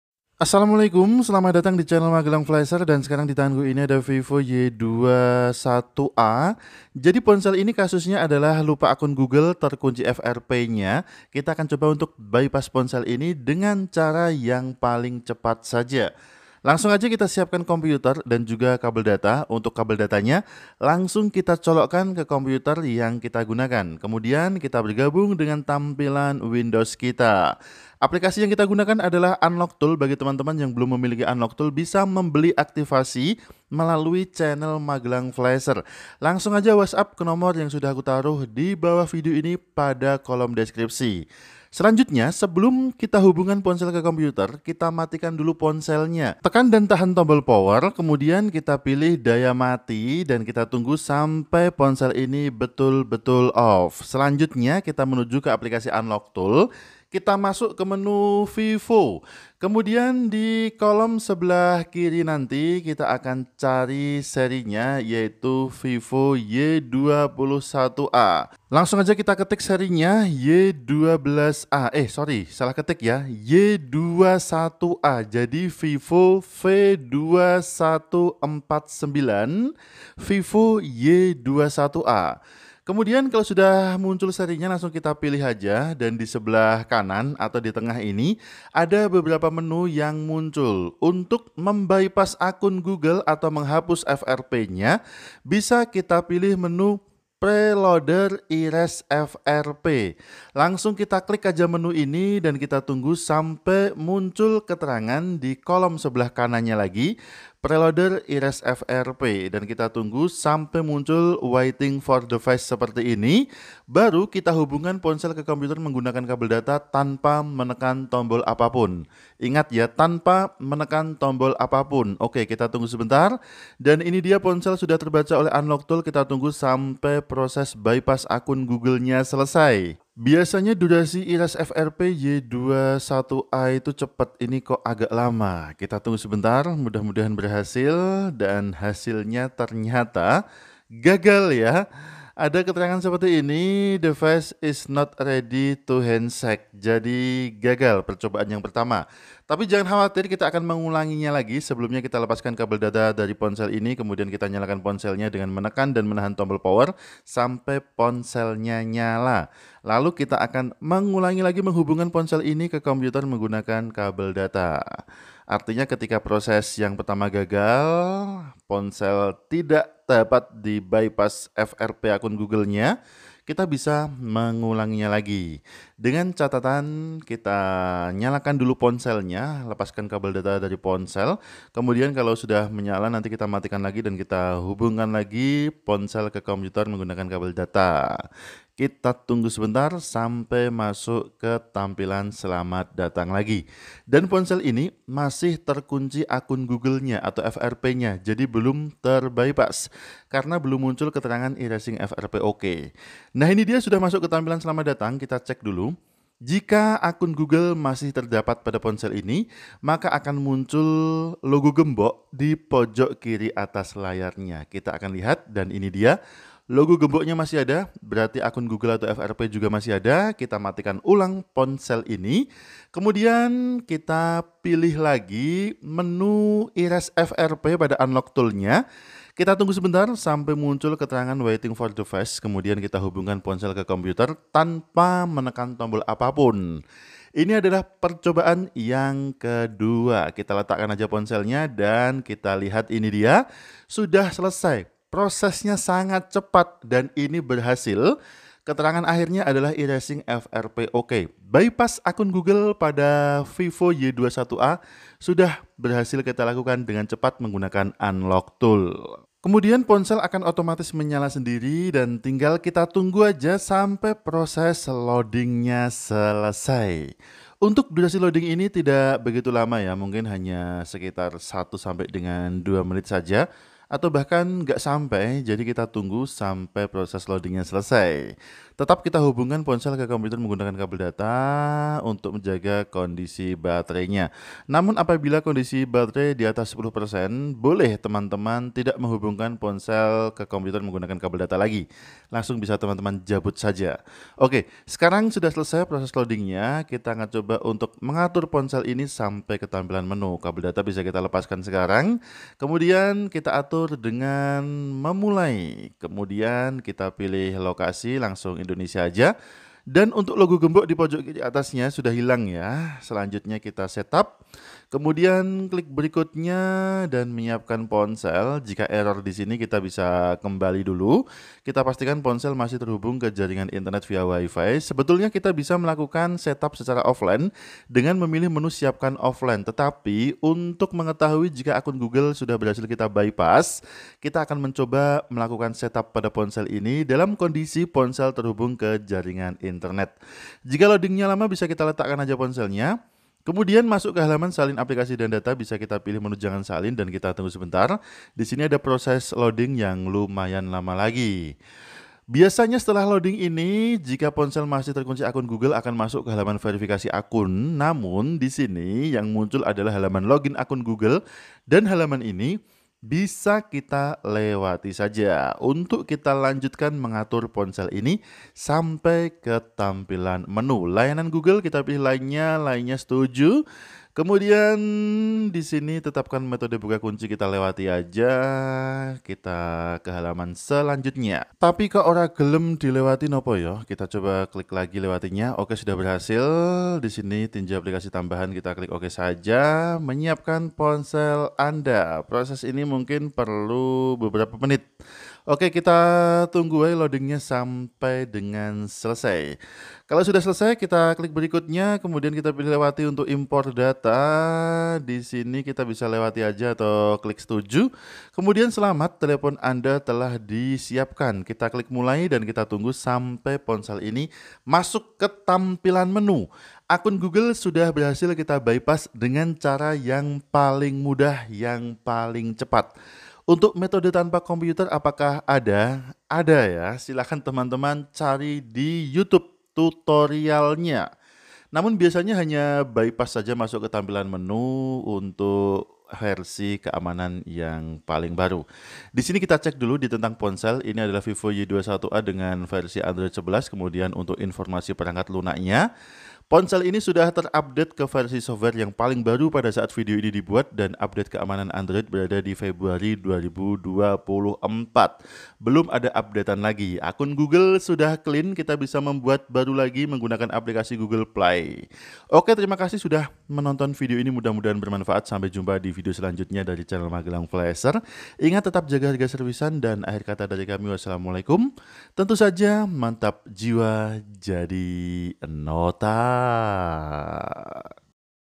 Cara Erase Frp Vivo Y21a Unlocktool | Vivo PD2139JF (v2149) Terkunci Akun Google
Cara Erase Frp Vivo Y21a Unlocktool | Vivo v2149 Terkunci Akun Google. Untuk aplikasi nya bisa beli melalui channel ini.